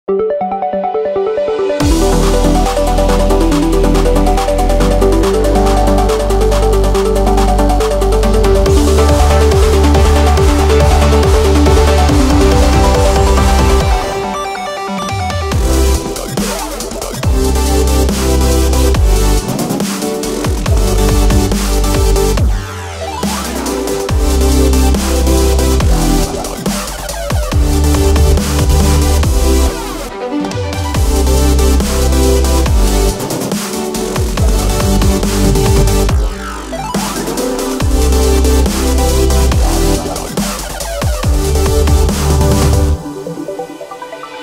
JENN LEE 2022秋冬時裝展/女團HUR參展短影@jennlee9949｜⚡HUR OFFICIAL
JENN LEE 22FW LOVE BUTTON HUNTER VR GAME EXHIBITION

Special thanks: @jennlee9949
這次我們女團HUR受邀參加了一場超酷的時裝展
以元宇宙的概念推出VR 互動虛擬遊戲
讓女孩們沉浸於設計師的世界之中
時裝展於 7/21到7/24
在華山1914文化創意產業園區的5B館
雷雷們快去膜拜一下吧!!!